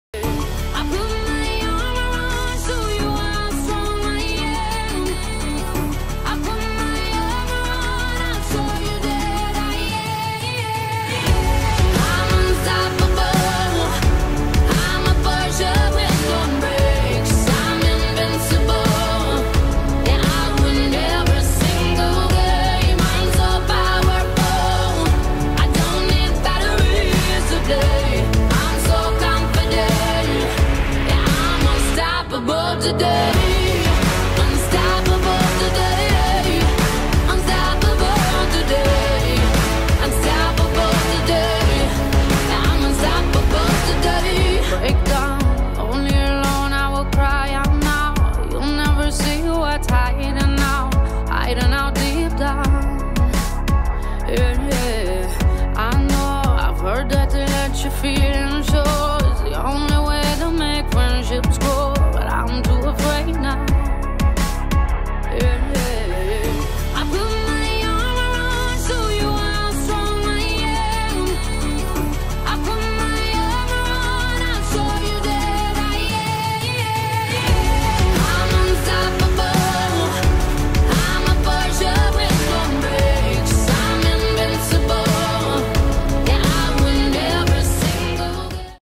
Suasana 2nd SEA OWS Championships Hari 1 dari Udara
Perjuangan para atlet di 2nd Southeast Asia Open Water Swimming Championships 2024, hari pertama, dilihat dari udara.